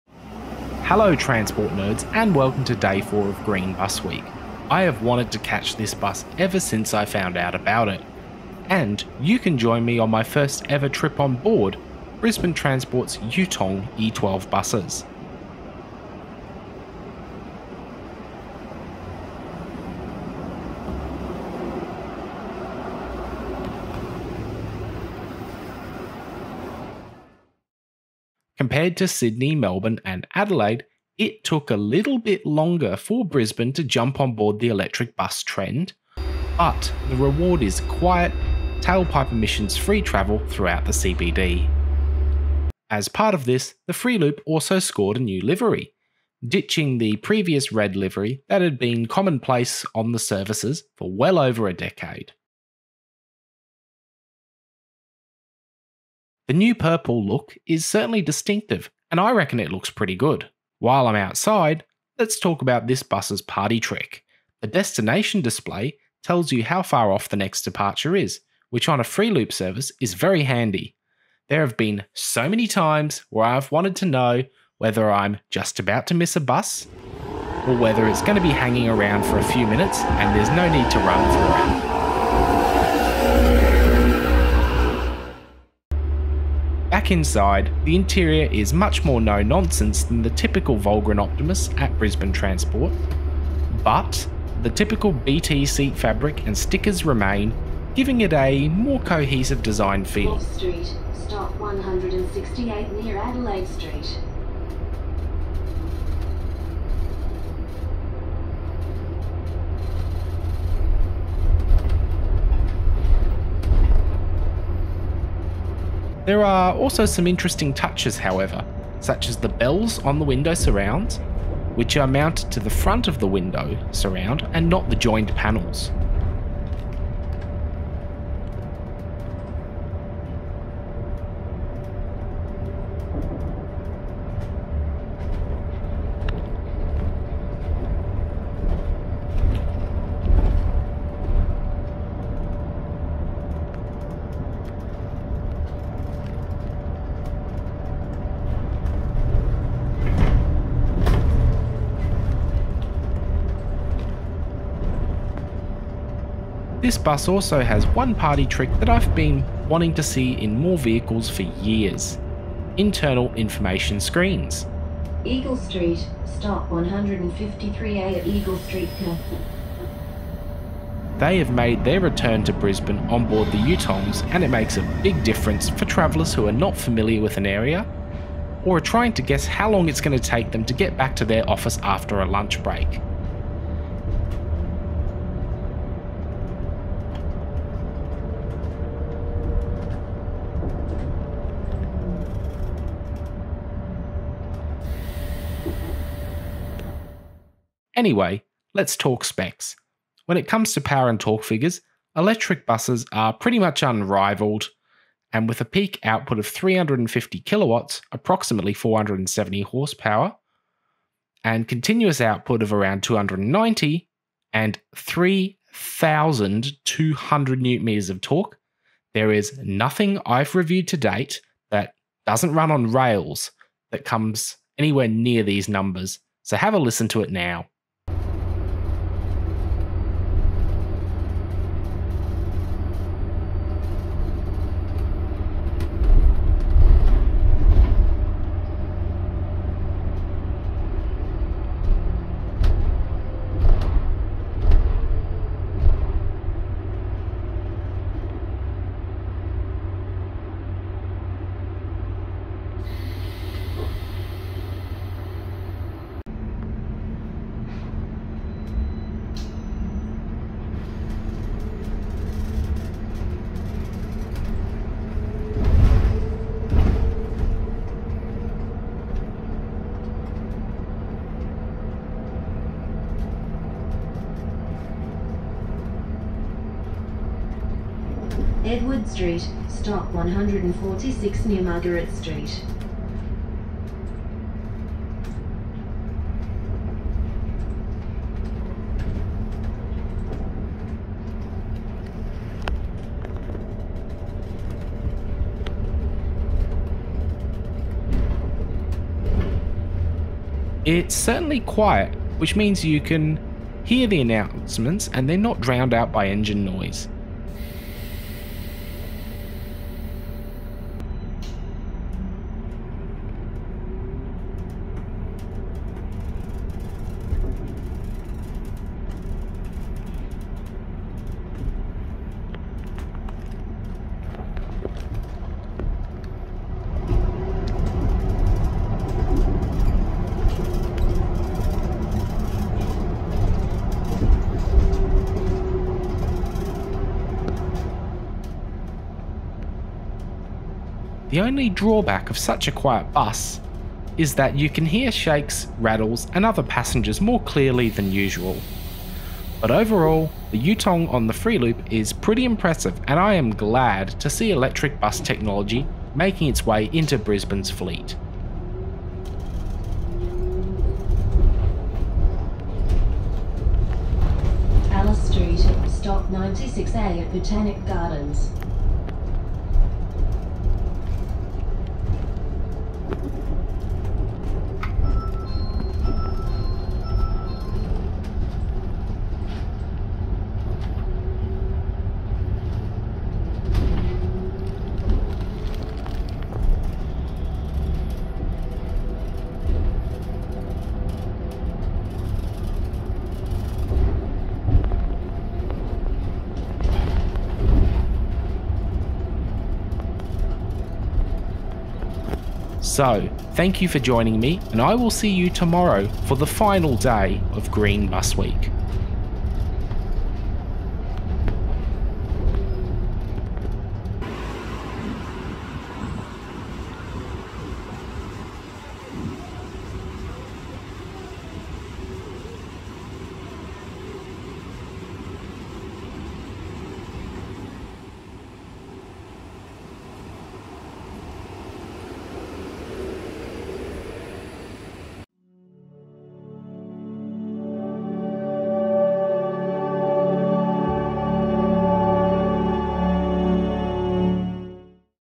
Green Busweek Day 4: Brisbane Transport Yutong E12
For day 4 of Busweek 8, I checked out a bus that I had wanted to catch as soon as I found about its existence.

Now, Yutong has been a common electric bus manufacturer in recent years and Brisbane chose to trial four of these on the Free Loop service. Having grown up with the red free loops for years, I was excited to see what the next generation of 40 and 50 services would be like.

Make sure to join me for the final day of Busweek 8  tomorrow!